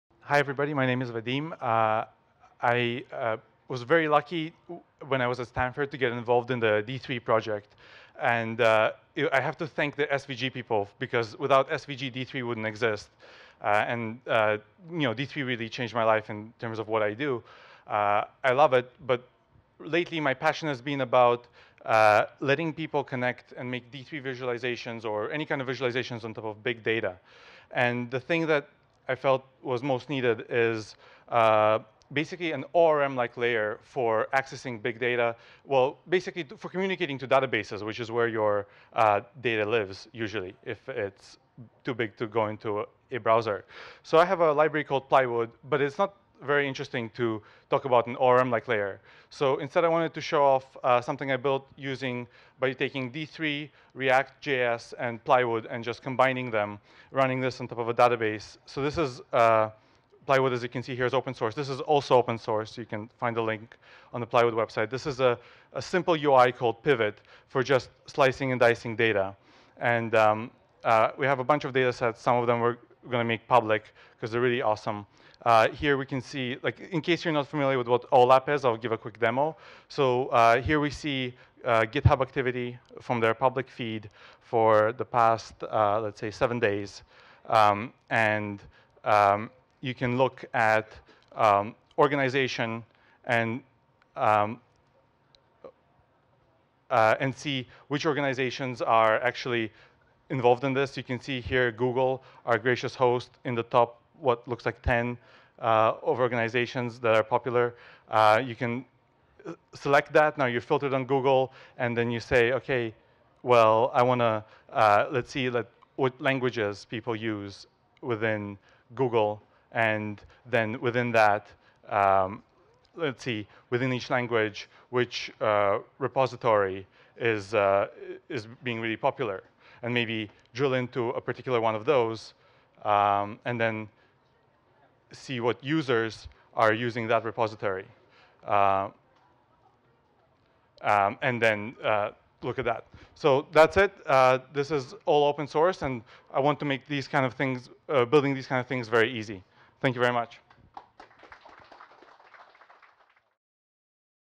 LIGHTNING TALK: Vadim Ogievetsky (Imply.io)
Vadim Ogievetsky (Imply.io)

Showing off Plywood, an ORM-like layer for OLAP data by demoing Pivot an open source OLAP explorer UI.